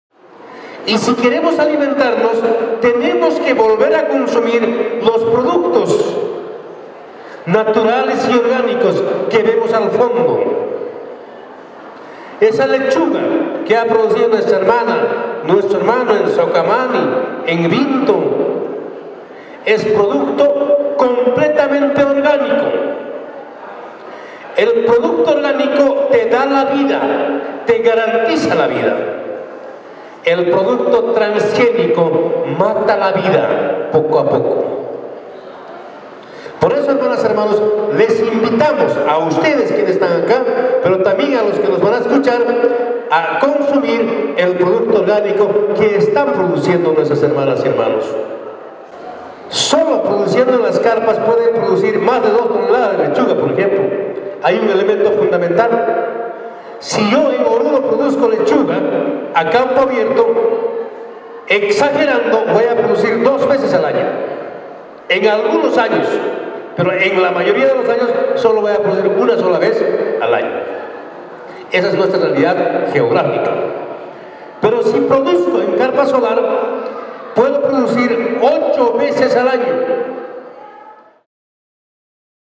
Lanzamiento AUP Oruro
El Gobernador de Oruro, Víctor Hugo Vásquez en el Lanzamiento del Programa de Agricultura Urbana y Periurbana mencionó la importancia de mantener una vida saludable consumiendo alimentos ecológicos.